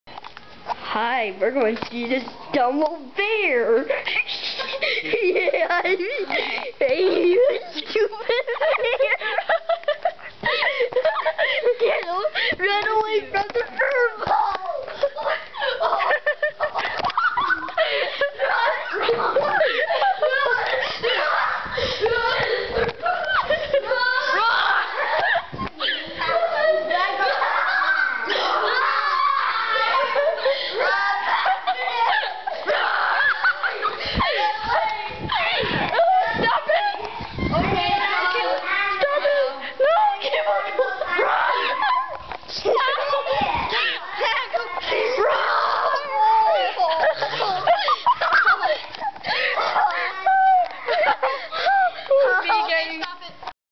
Dumb bear....
The person that played as a bear was Danny (a blonde person that was on my titanic vid) and he hurt us :(
R.I.P Ivy. :D and Ian